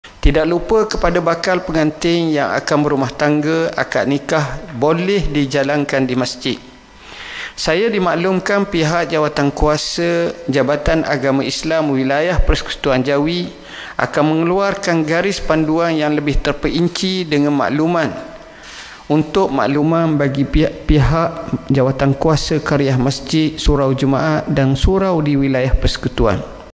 TERKINI | Akad Nikah Boleh Dilaksanakan Di Masjid
TERKINI | Aktiviti akad nikah kini boleh dilaksanakan di masjid dengan garis panduan yang terperinci yang akan diumumkan kelak.